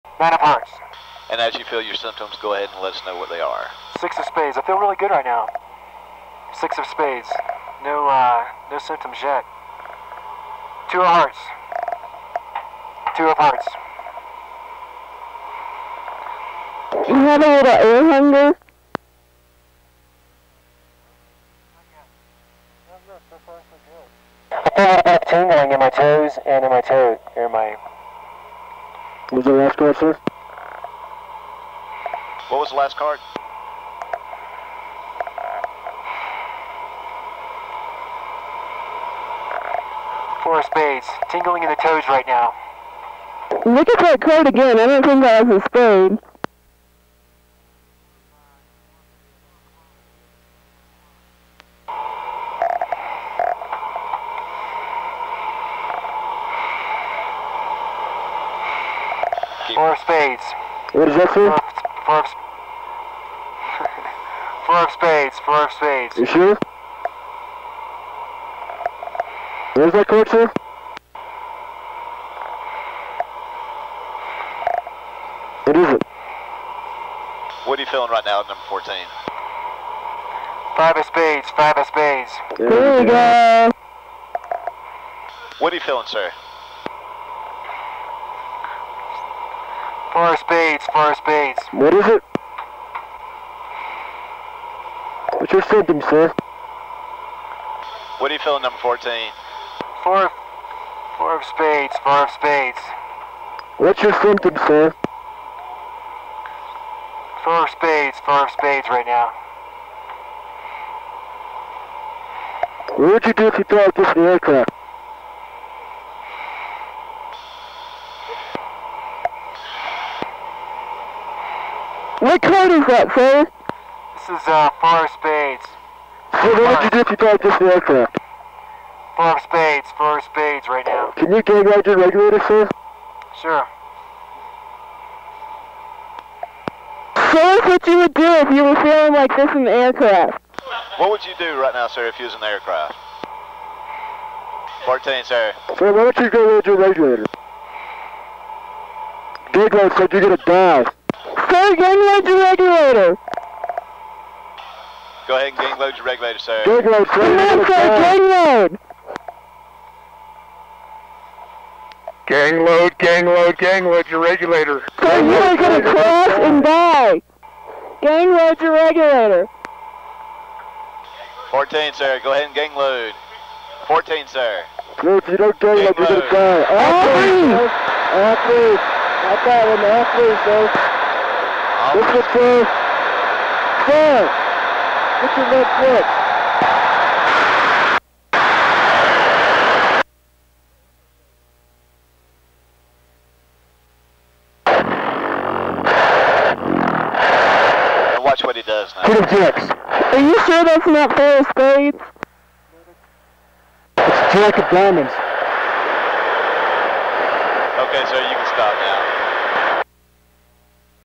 Funny Pilot confused with Hypoxia
Efeito da altitude. Falta de oxigenio no sangue. Hipoxia.